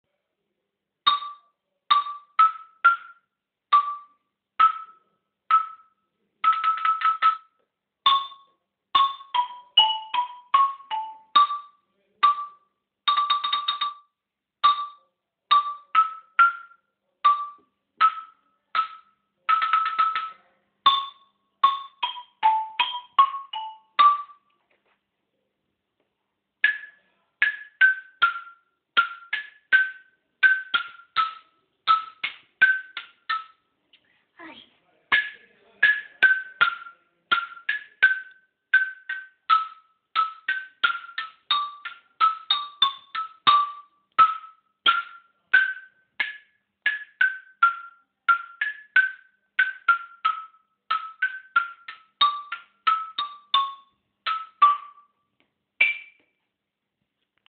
Ensayo de Sergio Fernandez (Alumno de la Escuela Municipal de Musica de Beas)
En este video podemos ver las cualidades de Sergio Fernandez con el Xilofono. Año 2008